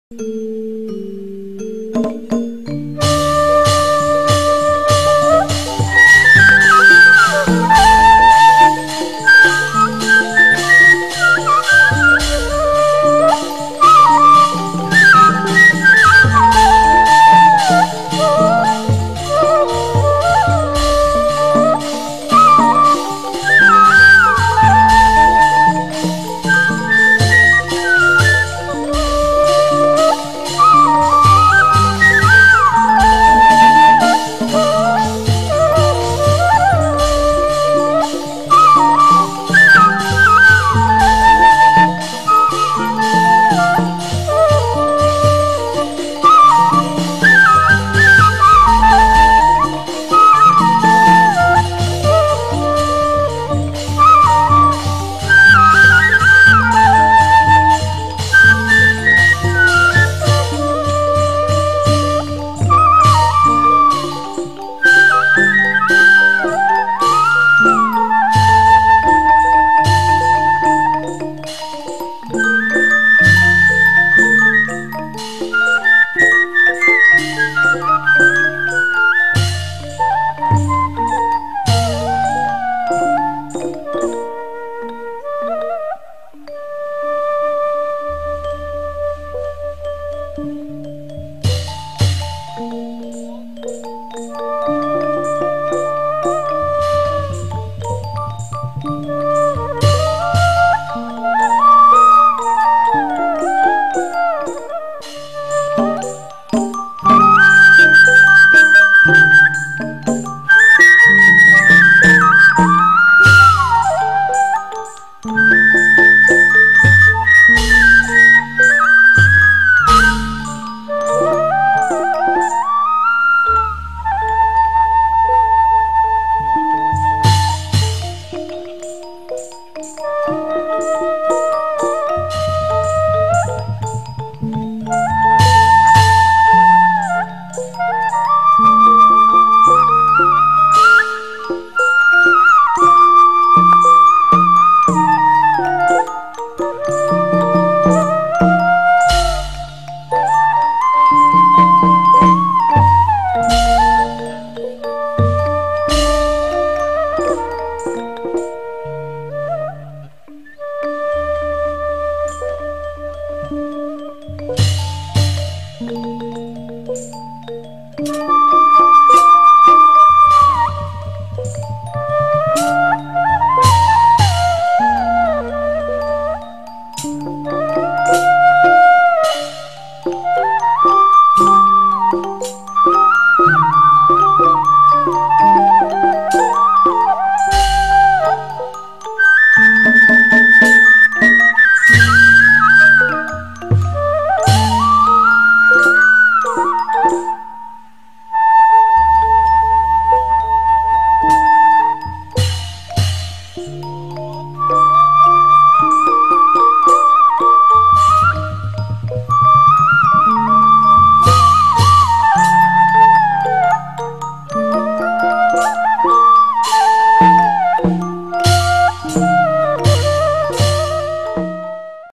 TALUAN SINGA DANGDUT PUTRI ANDINI
Dalam Rangka Pesta Perhajatan Kel. Bpk. Kuwu Tanari - Ibu Titin Supriantin
Ds. Kediri - Wates Kec. Binong
17 September 2017